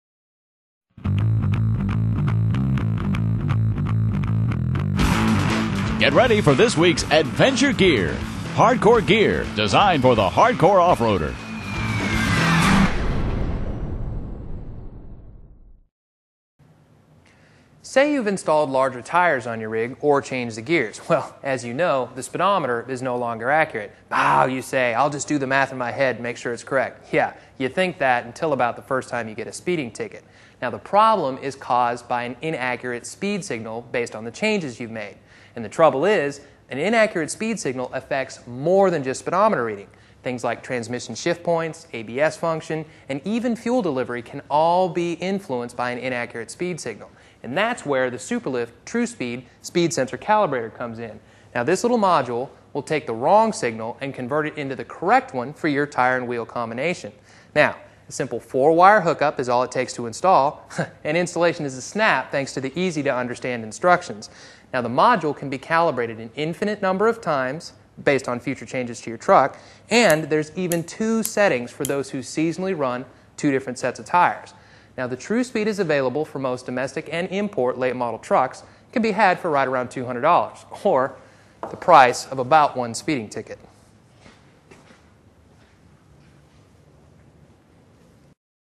Superlift TruSpeed Speed Sensor Calibrator
Speed Sensor Calibrator Truspeed - Virtually every vehicle manufactured since 1992 is equipped with an electronic speedometer that uses a sensor to read vehicle speed rather than a mechanical cable. The speed signal is also a big factor in the overall performance equation. When tire height and / or axle gearing is altered, the vehicles Engine and Powertrain Control Modules receive an erroneous speed signal. This translates into subtle, yet real reductions in performance and fuel economy.

Our TruSpeed calibrator addresses all speed signal-related problems; it doesnt just correct the speedometer. How? The TruSpeed module installs in-line with the factory electronic speed sensor and converts the wrong speed signal into the correct one for your vehicles new tire and / or axle gear ratio combination. Because the signal is altered at its source, all speed-sensitive vehicle systems will receive the correct speed data, which restores all proper functions.